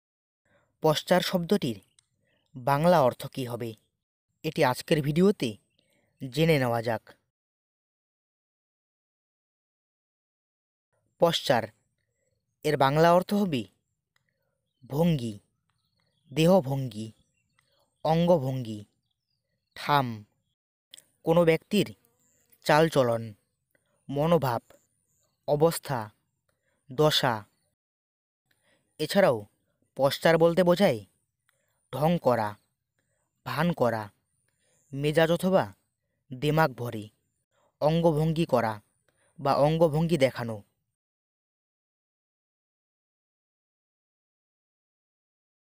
Posture Meaning in Bengali || Posture শব্দের বাংলা অর্থ কি || Bengali Meaning Of Posture
এই ভিডিওতে posture শব্দের বাংলা অর্থ নিয়ে আলোচনা করা হয়েছে।

posture শব্দের বাংলা অর্থ কি?